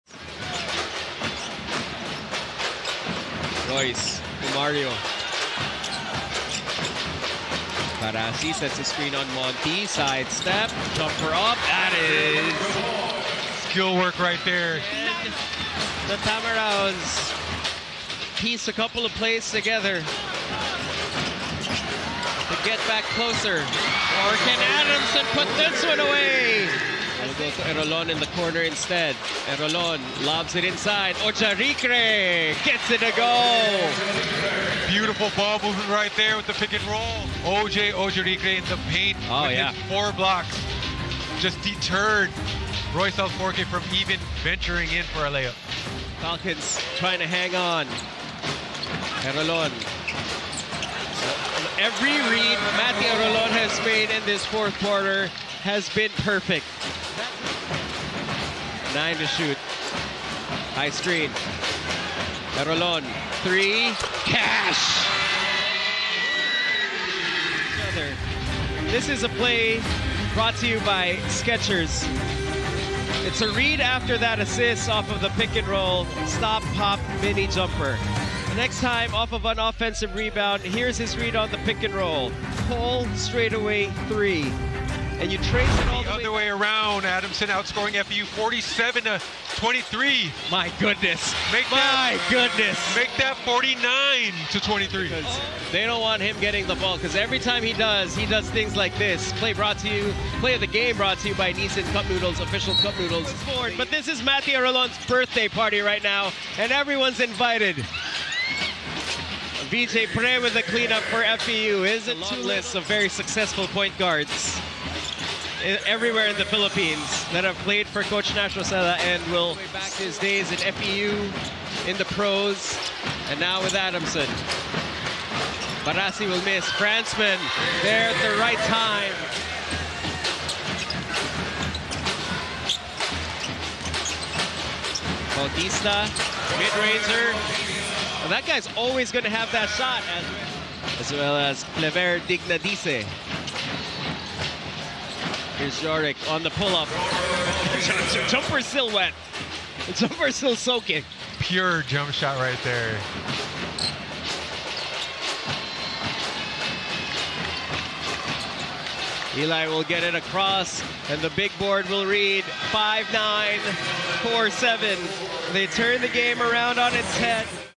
AdU vs FEU | 4TH QUARTER GAME HIGHLIGHTS | UAAP SEASON 87 MEN’S BASKETBALL | SEPTEMBER 8, 2024
WINGED VICTORY ❤️‍🔥

The Adamson Soaring Falcons prevail as they come from behind for win no. 1 in UAAPSeason87 against the FEU Tamaraws!